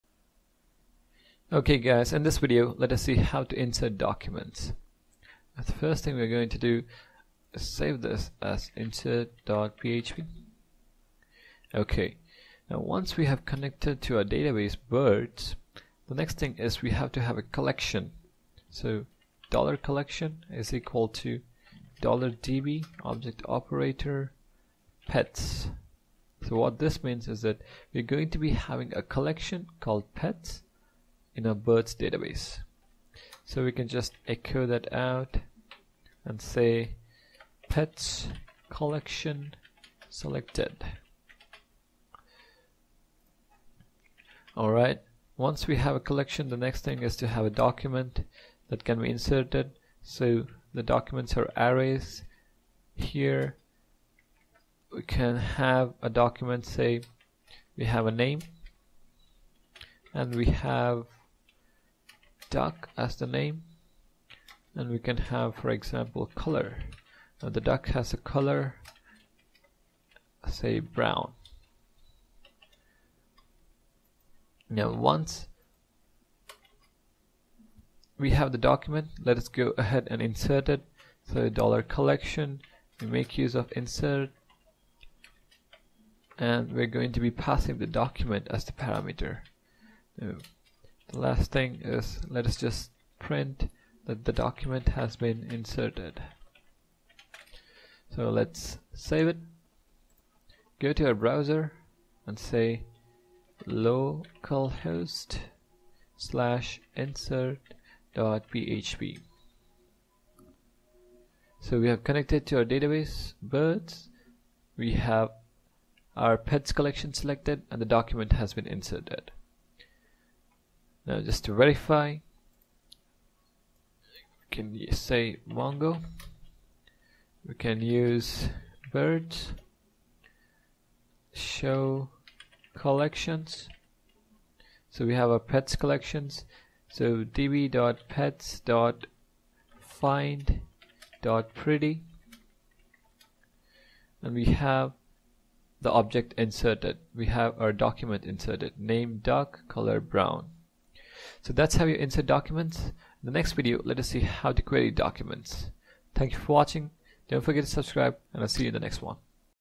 MongoDB Tutorial for Beginners - 21 - MongoDB PHP(2/5)
Insert into MongoDB with PHP.

Mongodb |Mongodb Tutorial | Mongodb PHP | PHP Mongodb Insert